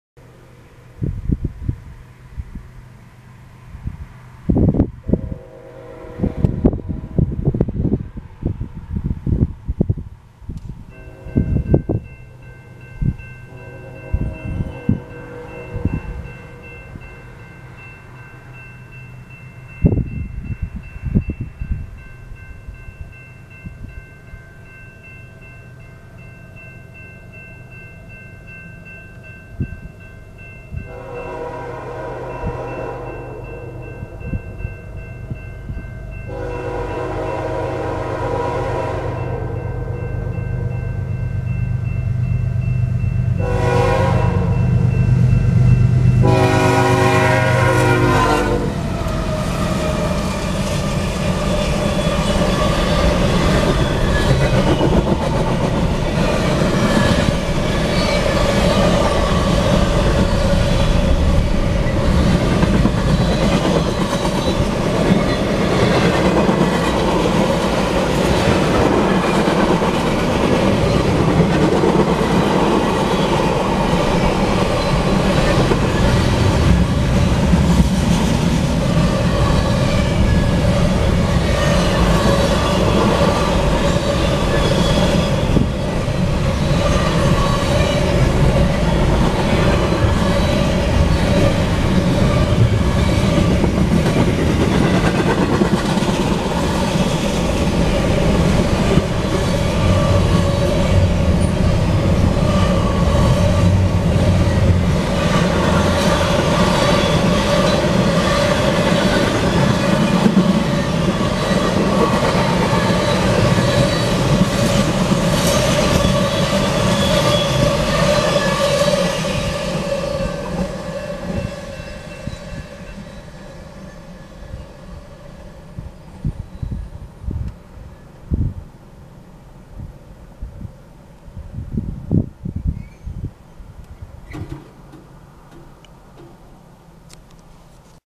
BNSF 7657 East ZSTPCHC (2-16-2018)
CP 9551 (GE AC44CW)
Z St. Paul-Chicago/Main Track 2/M.P. 393.26